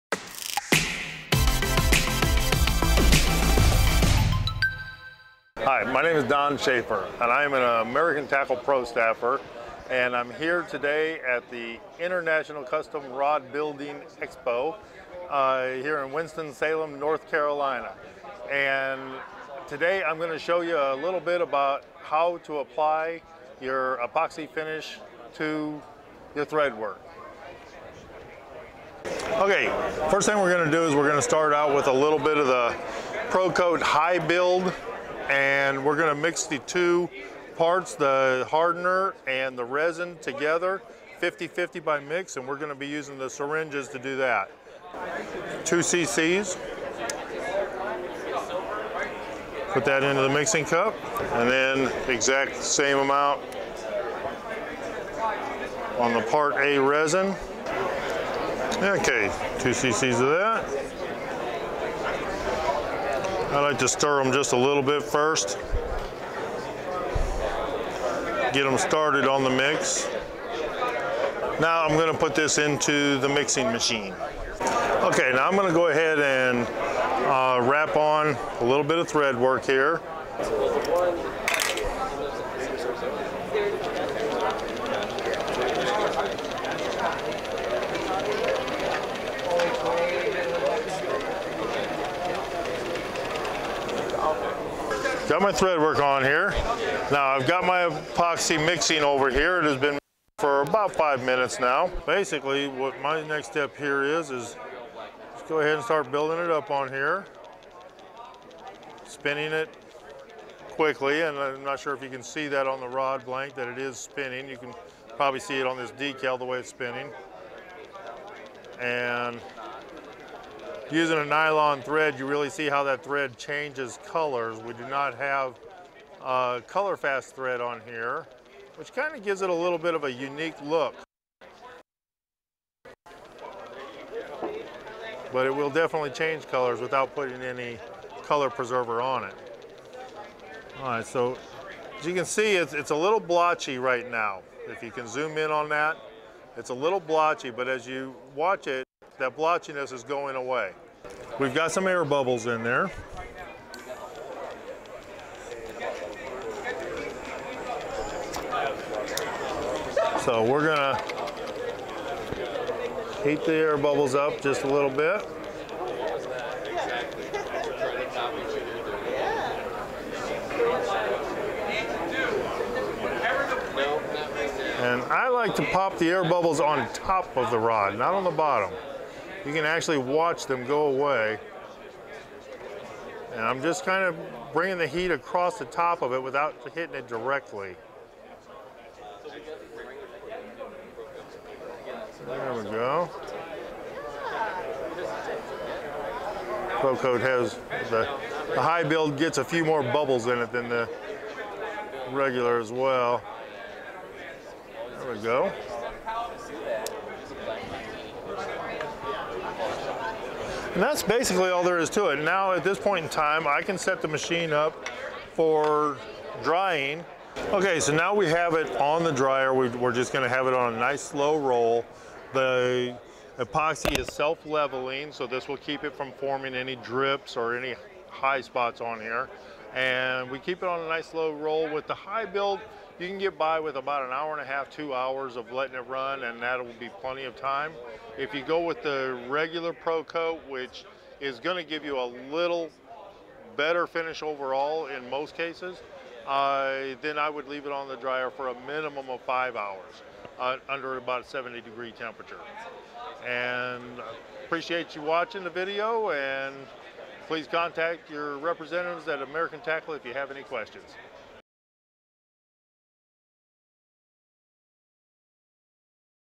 AT ProStaff Rod Building Tips with Don Shafar applying Epoxy Finish
On this episode of AT ProStaff Rod Building Tips we chat with Don Shafar of Backbone Custom Rod company about the best way to apply epoxy finish to your rods. Filmed on location at ICRBE.